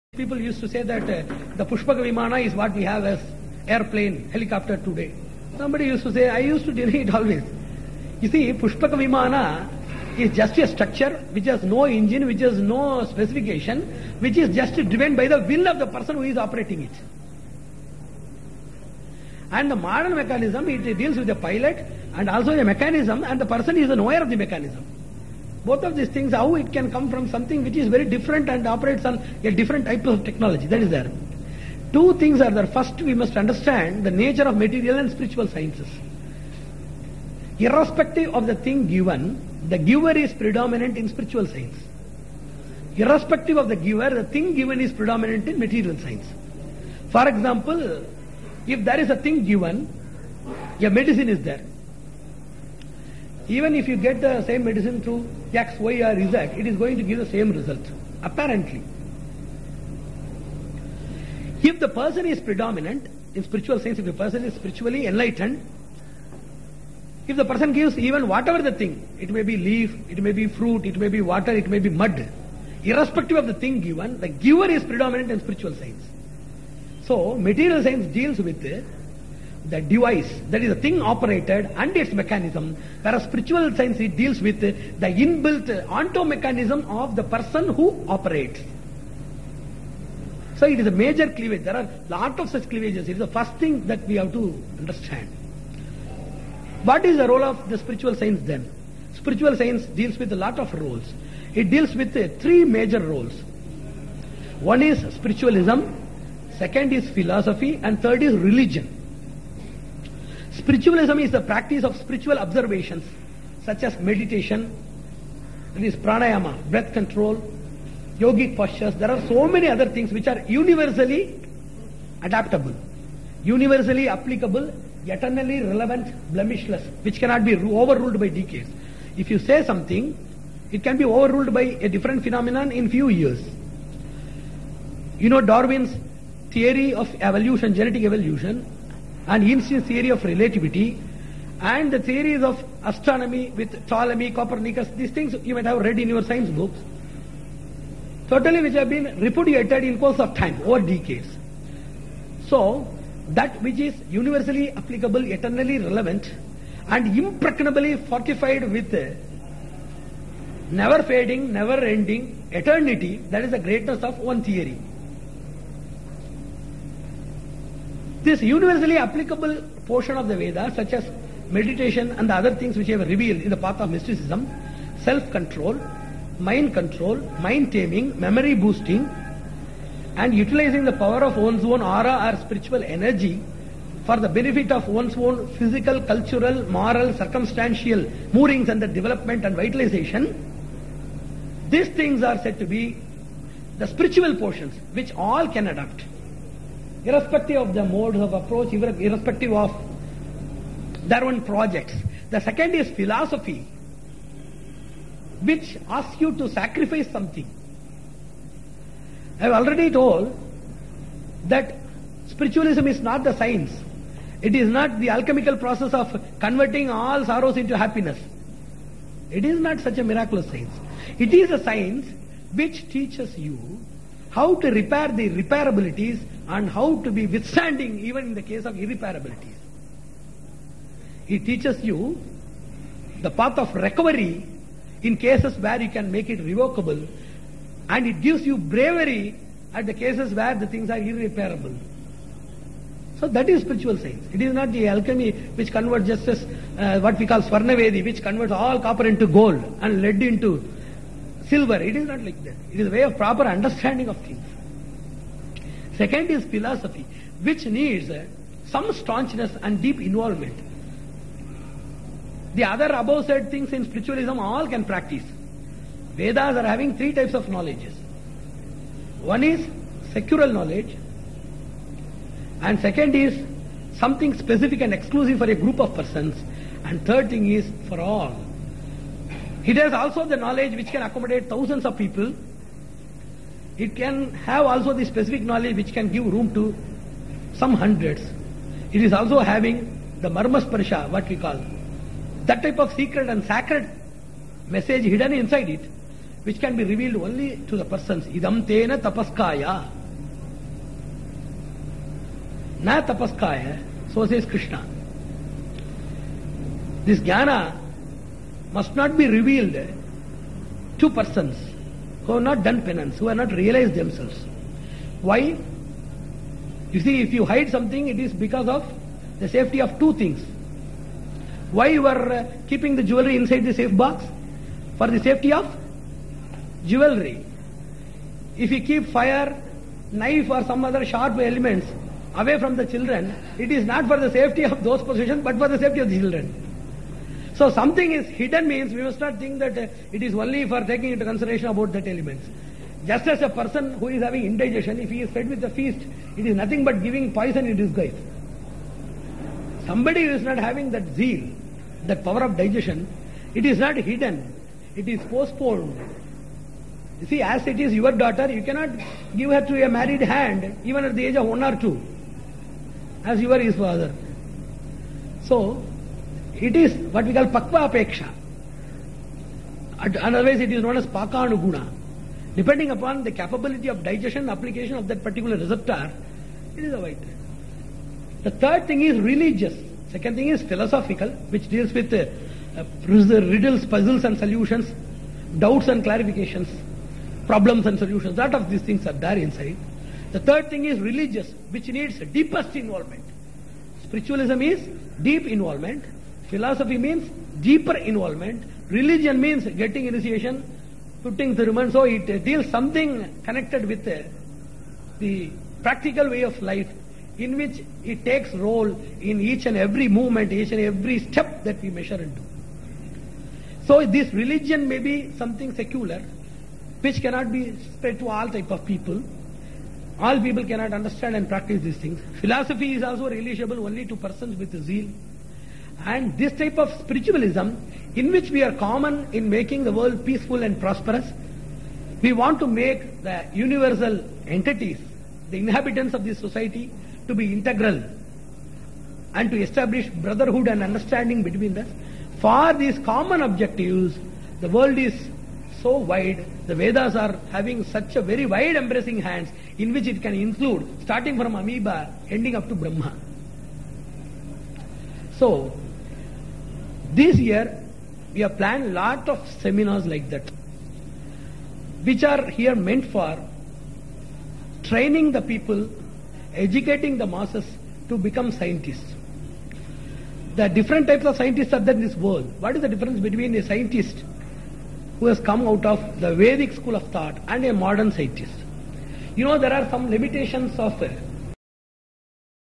Wisdom 8
A power packed English by Sri U.Ve Prasanna Venkatachariar Chaturvedi Swami on the topic "Wisdom"